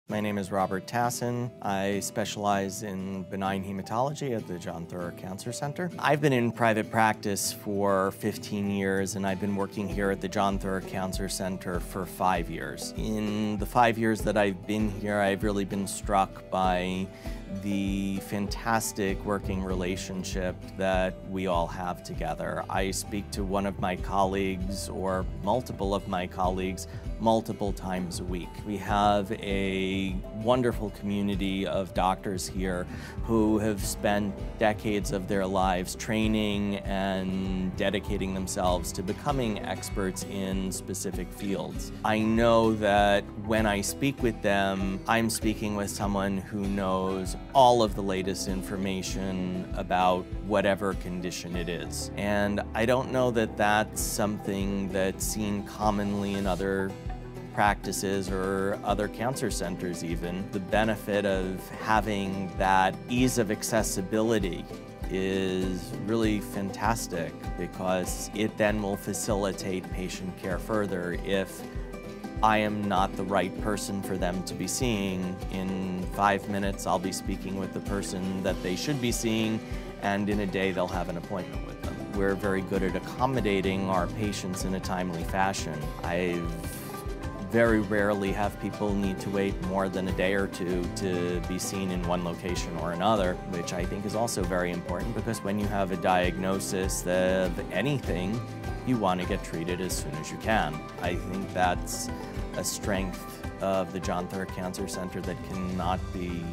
Robert Tassan, M.D., specializes in the broad field of benign hematology
Dr. Robert Tassan specializes in the broad field of benign hematology at John Theurer Cancer Center, which includes non-malignant blood disorders such as anemia, bleeding disorders, thrombosis, or overproduction syndromes. Dr. Tassan especially values the teamwork amongst his peers, which provides his patients with easy access to the expertise they need to manage their condition.